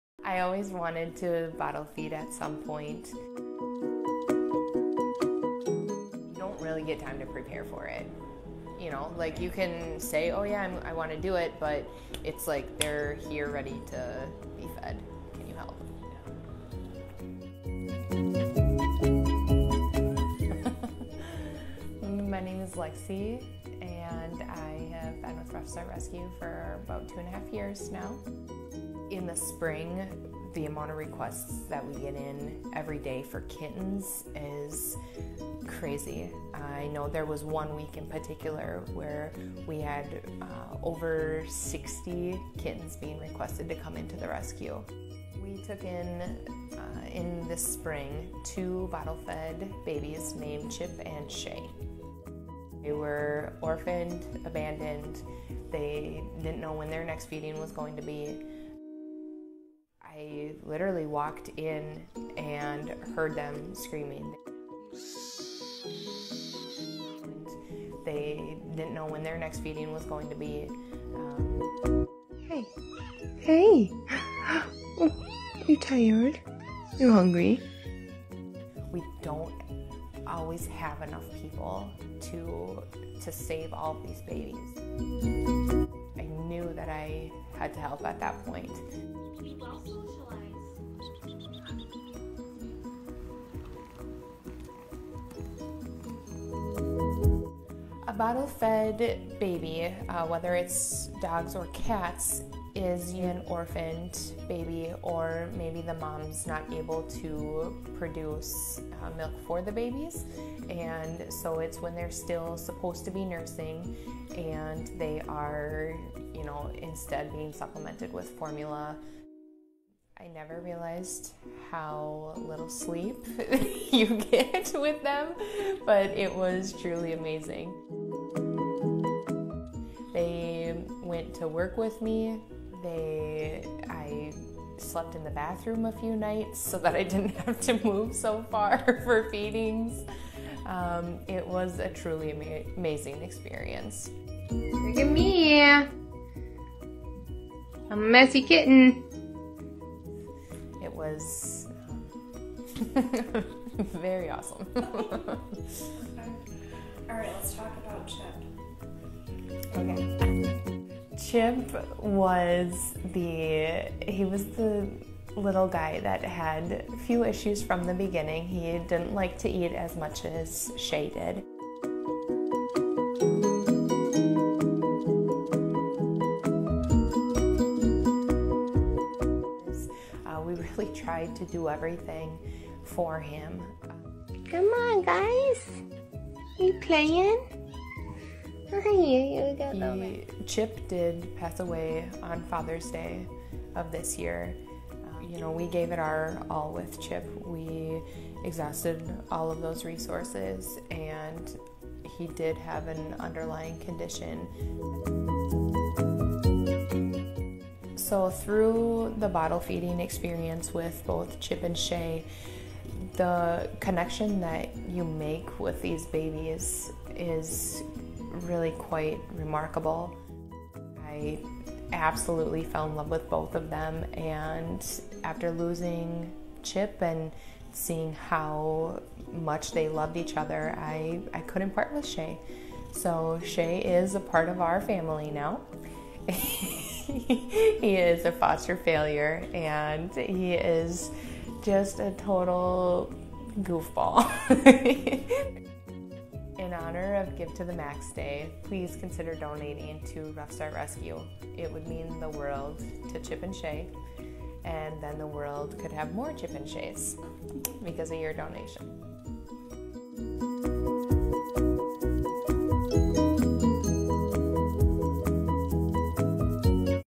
Bottle Fed Kitten Story
Some of the tiniest animals leave the biggest impact on our hearts and memories.
Kittens and puppies are very fragile in the first few weeks of life. Mortality rates can be as high as 15%. When a mom or a surrogate are not available, our foster families step up to be the parent for these animals. Bottle feeding can be a substantial time commitment and a great deal of patience is needed. Contributions ensure we have the necessary supplies to help our babies grow and thrive.
Your support gives us the resources to give these babies the best chance at long and healthy lives.